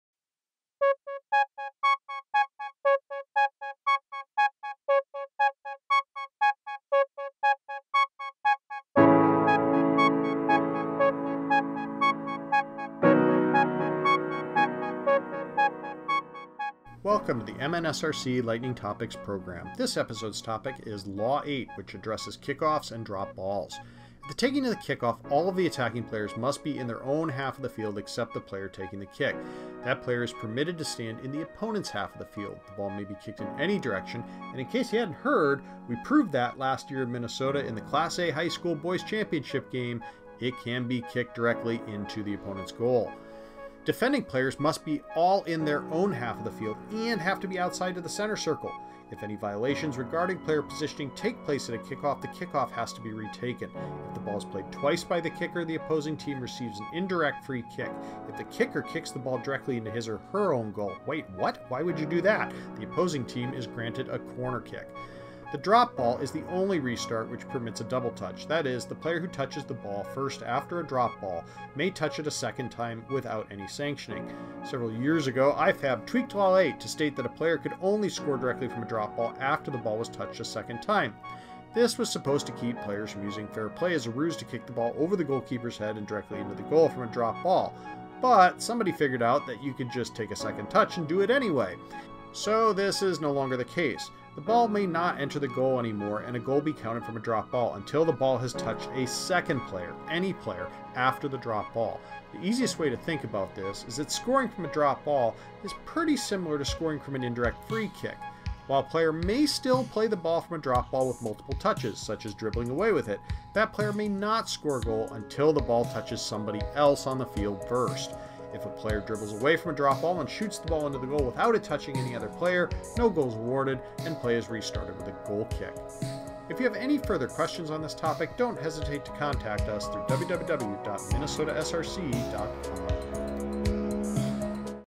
MNSRC Lightning Topics, Series 1, Episode 4: Law 8, Kickoff and Dropped Ball
This MNSRC Lightning Topic looks at Law 8 covering kick-offs and dropped balls